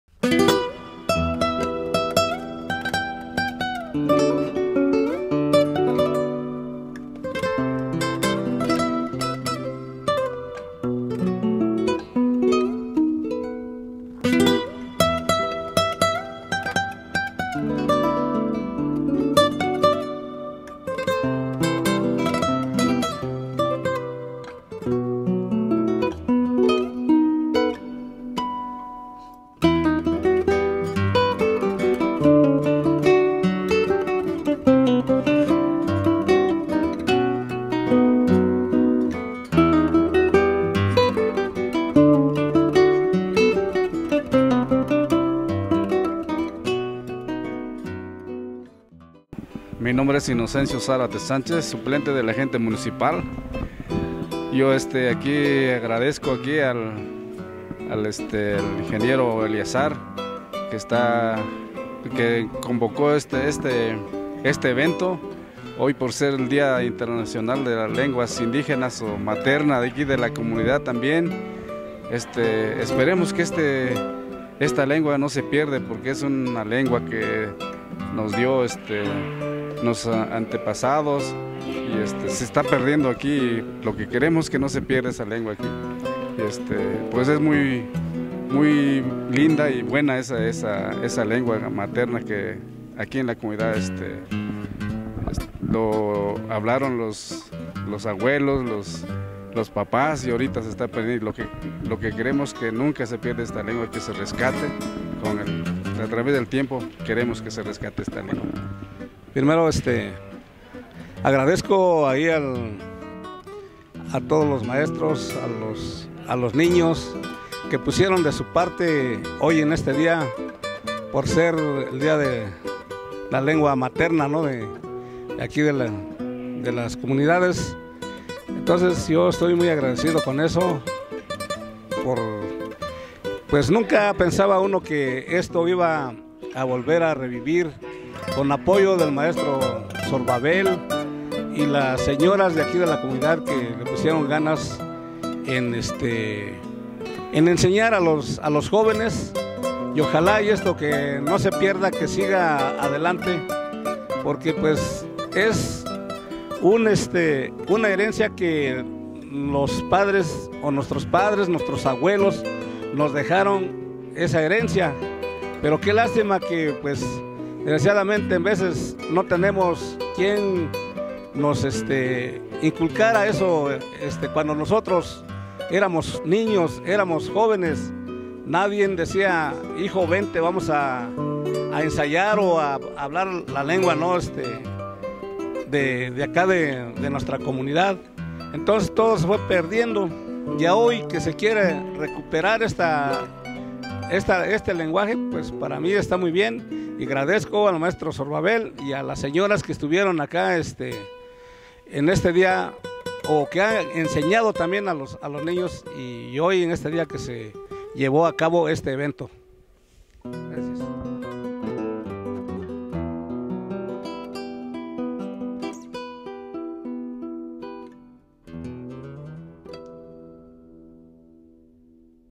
SAN PEDRO EL ALTO LENGUAS MATERNAS
21 DE FEBRERO SE CELEBRA DIA INTERNACIONAL DE LAS LENGUAS MATERNAS
SAN PEDRO EL ALTO REALIZA ESTE TIPO DE ACTIVIDADES
EN SU LOCALIDAD
RECUERDEN GRABAMOS TODO TIPO DE EVENTO